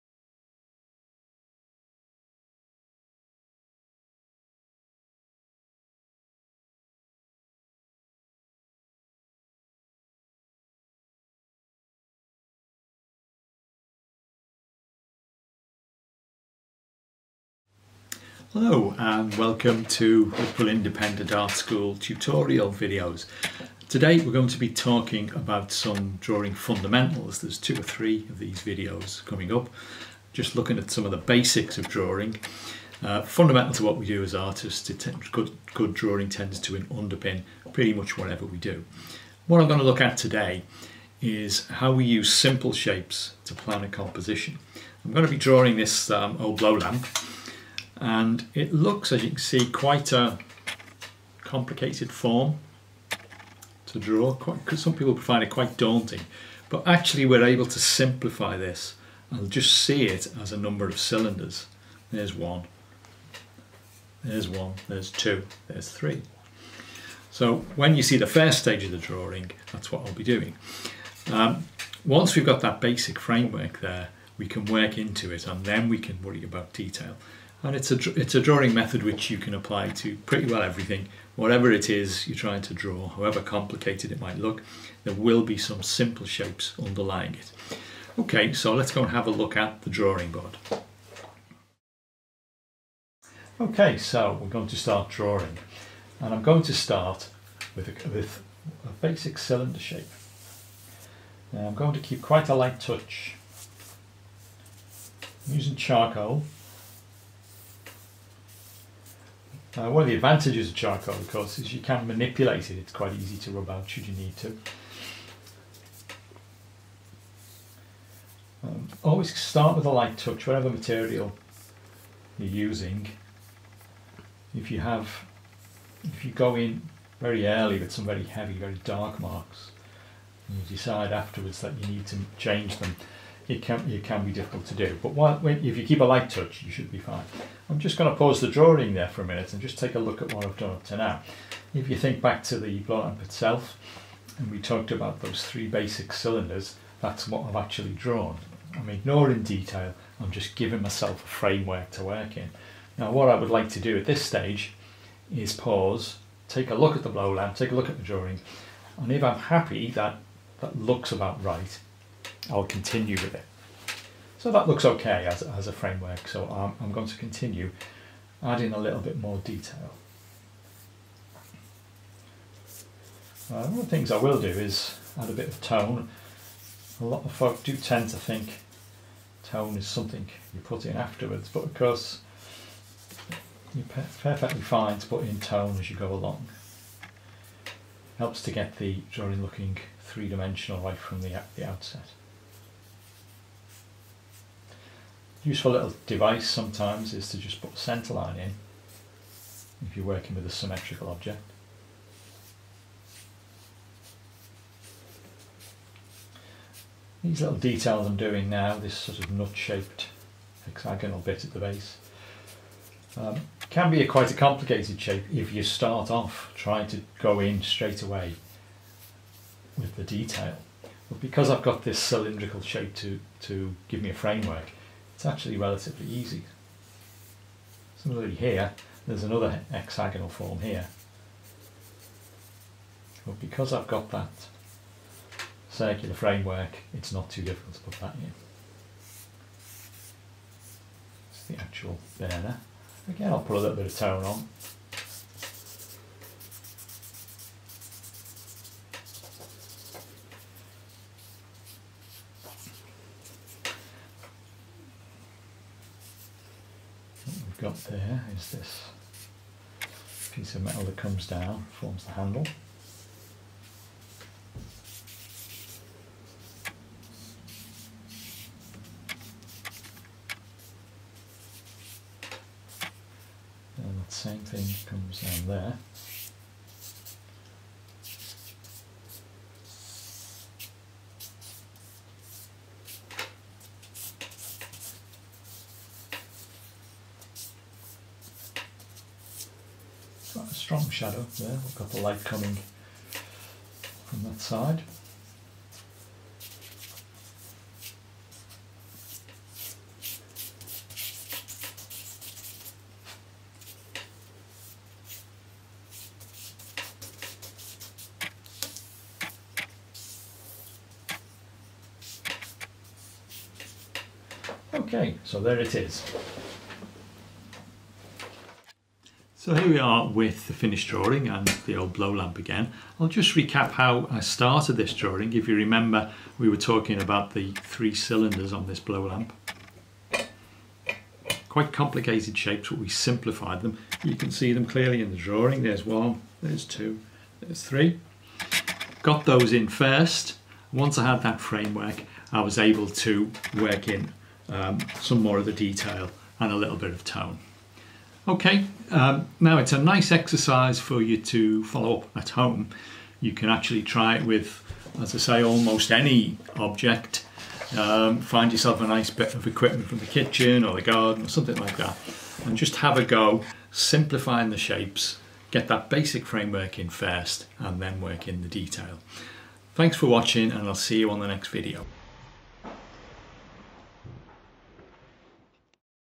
Drawing Fundamentals 1
This is the first of our videos where we break down various underlying ideas in drawing that can help build your confidence and skills quite quickly